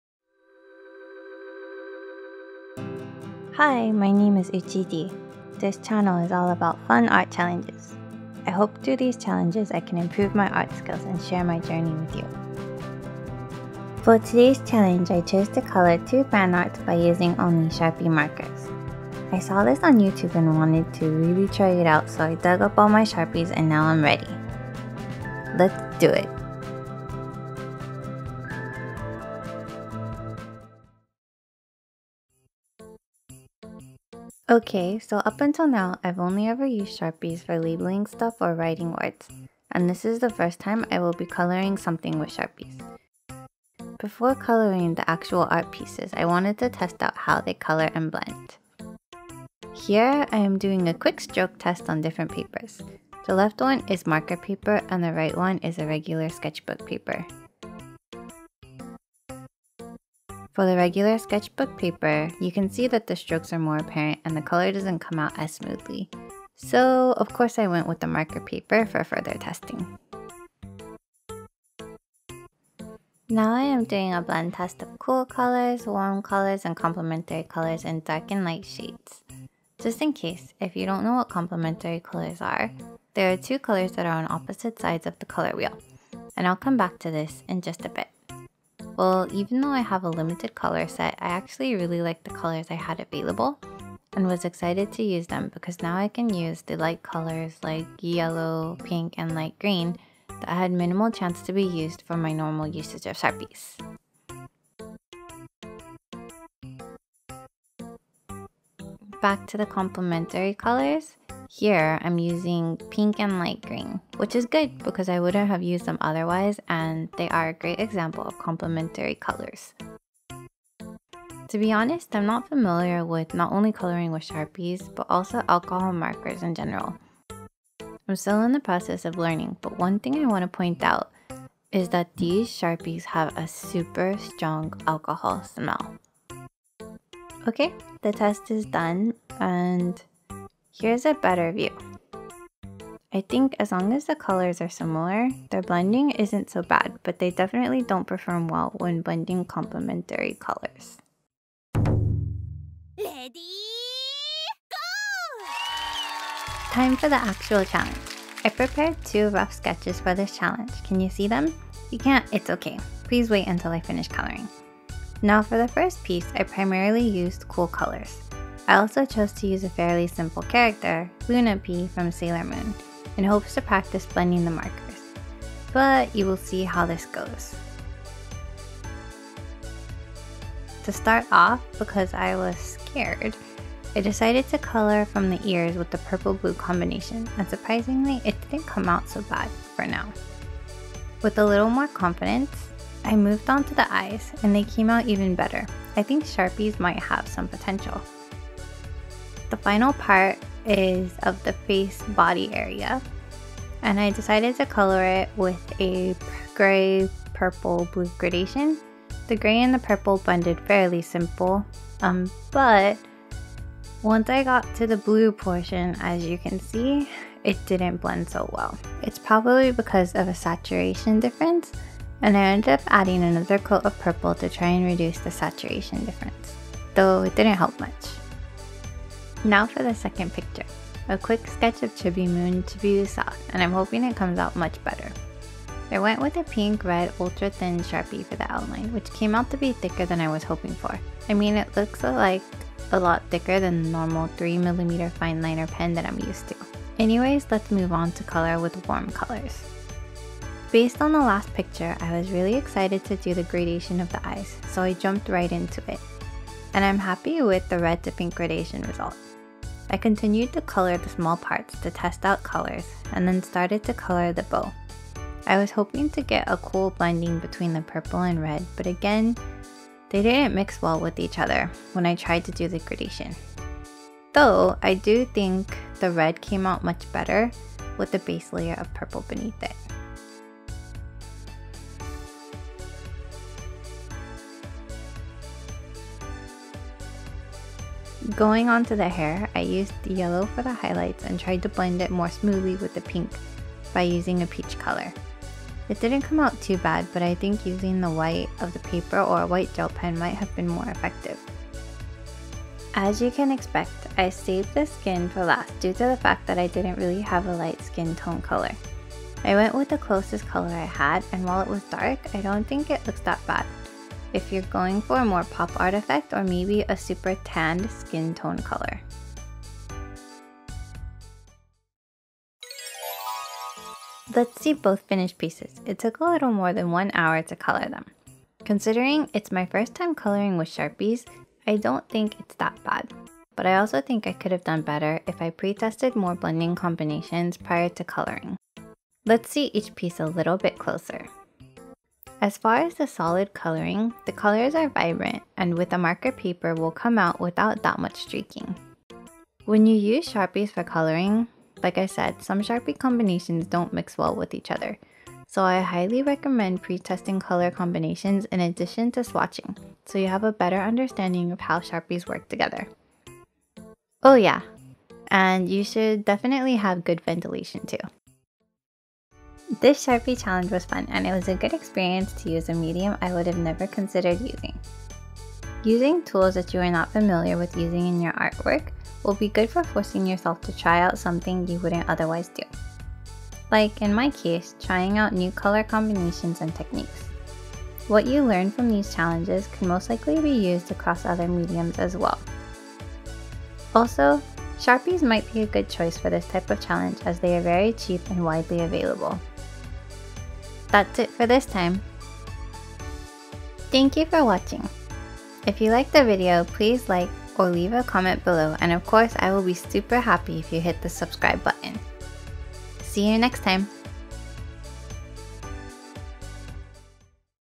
Only Using Sharpies to Color a Fanart | Sharpie Challenge
Here's my 2nd video! I finally got some use out of my old sharpies but sorry the sharpie smell must have affected my voice as the volume of my voice is low XD
I'll be sure to work on the volume balance in the next video!!

0:00 Introduction
0:32 Sharpie Color Test
2:31 Drawing with Sharpies
5:22 Sharpie Review
6:08 Challenge Summary